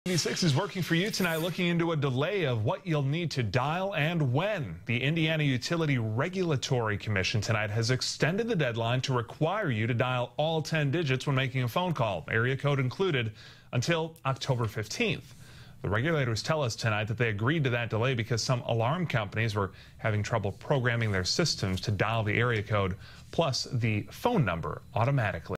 Ten-digit dialing delayed by regulators
You must dial all 10 digits starting October 15.